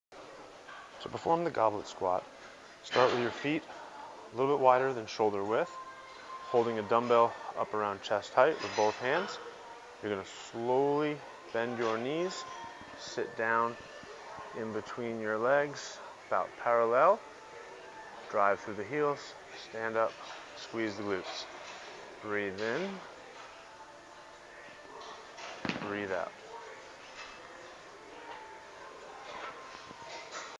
Goblet Squat Demo - iPersonalTrainOnline.com
1)Stand holding a light kettlebell by the horns close to your chest. This will be your starting position.
2)Squat down between your legs until your hamstrings are on your calves. Keep your chest and head up and your back straight.
3) At the bottom position, pause and use your elbows to push your knees out. Return to the starting position, and repeat for 10-20 repetitions.